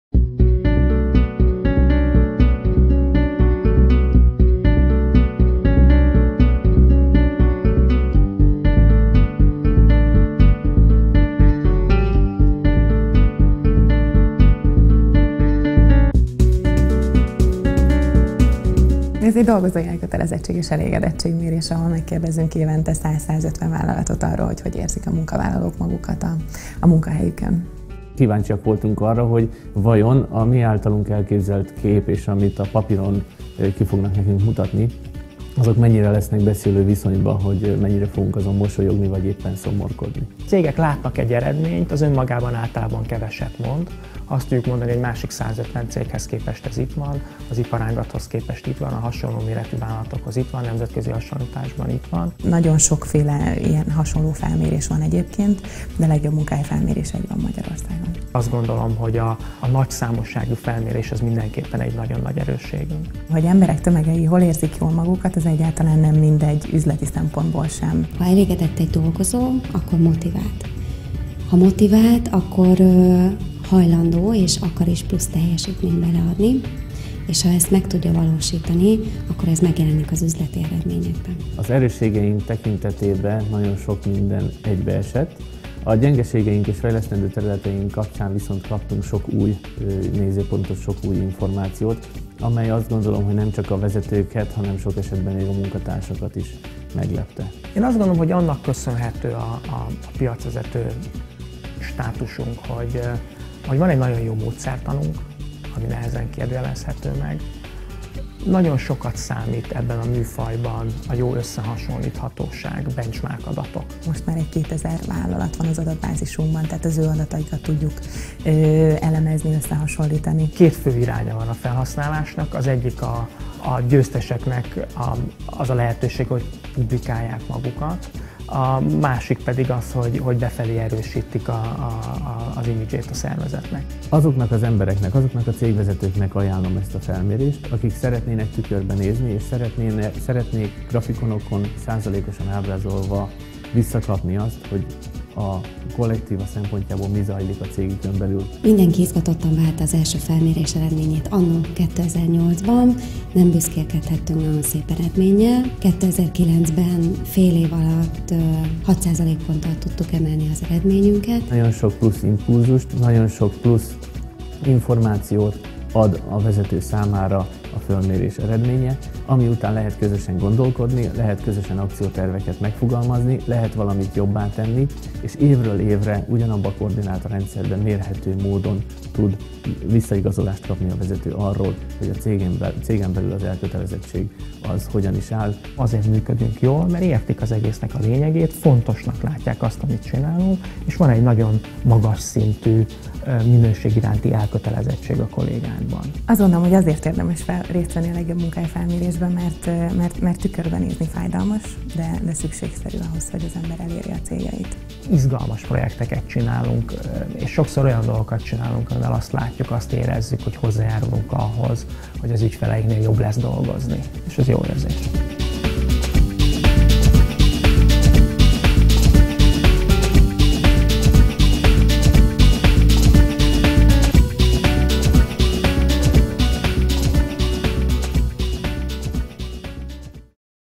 Aon Hewitt - Legjobb Munkahely Felmérés
A Legjobb Munkahely Felmérés Magyarország legnagyobb múltra visszatekintő, átfogó kutatása. A Felméréssel ügyfeleinket komplex módon, a fenntartható üzleti sikerek megteremtésében támogatjuk. A kutatás központjában az elkötelezettség mellett a vonzó munkáltatói arculat, a hiteles és felkészült felsővezetés, a teljesítményorientált kultúra és a fenntarthatóság szempontjai állnak.